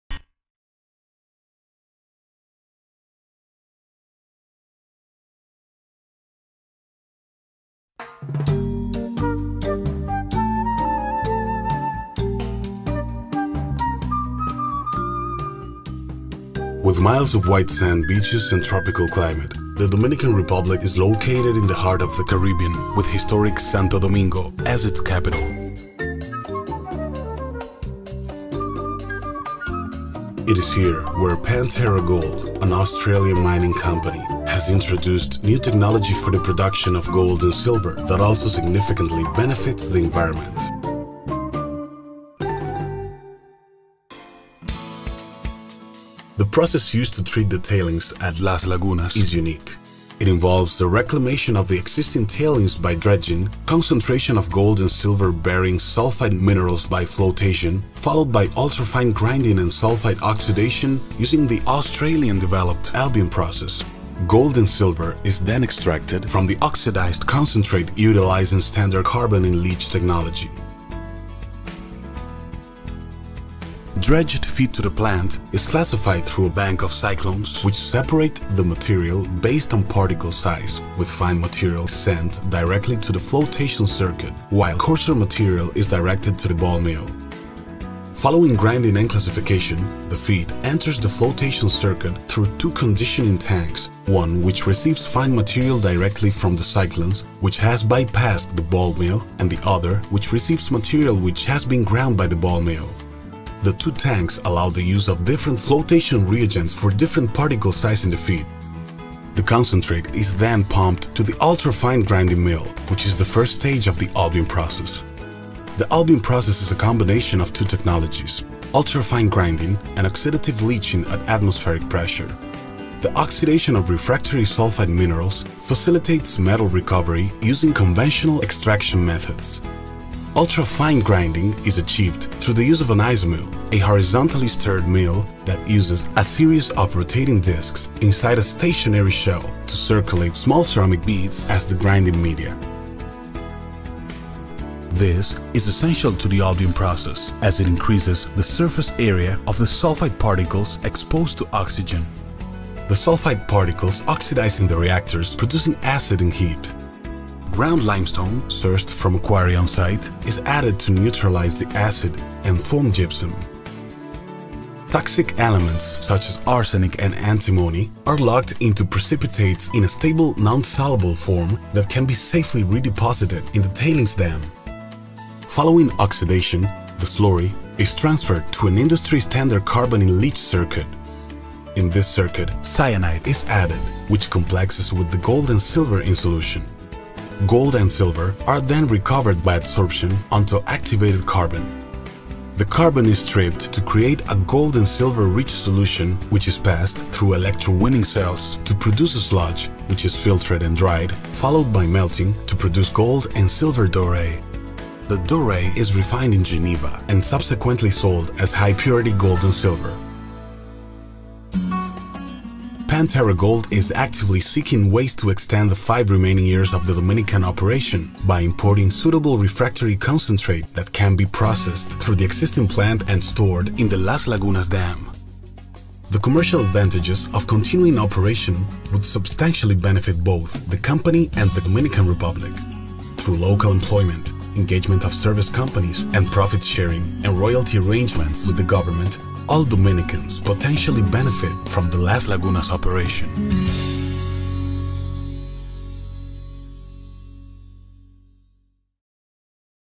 Albion Process at Panterra Gold
Albion Process description at Panterra Gold's operation in Dominican Republic (full video available on Panterra Gold's website, owned and produced by Panterra Gold)

Do you have a question for our team?